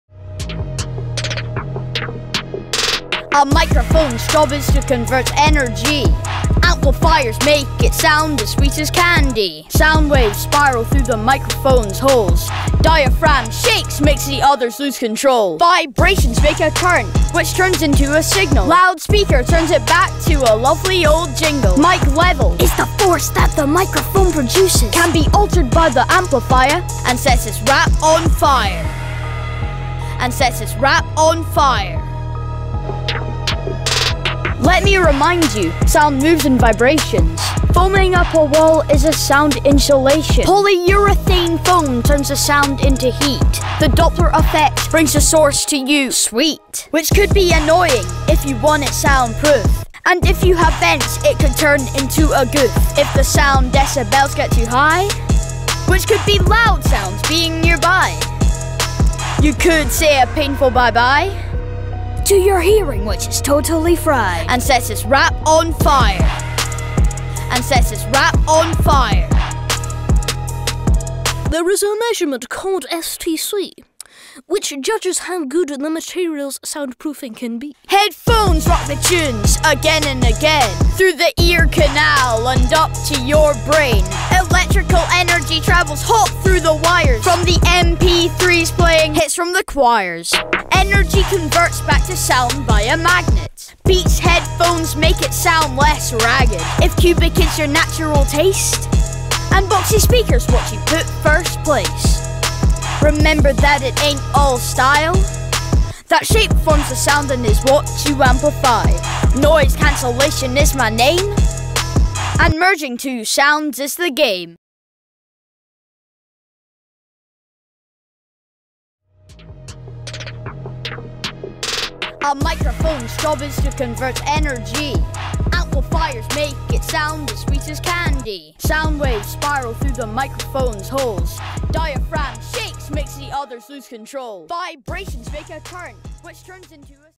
The Physics of a recording studio RAP!
Hi - here's my entry for the Science Project - hope you like it! Please do not show this to the school or class.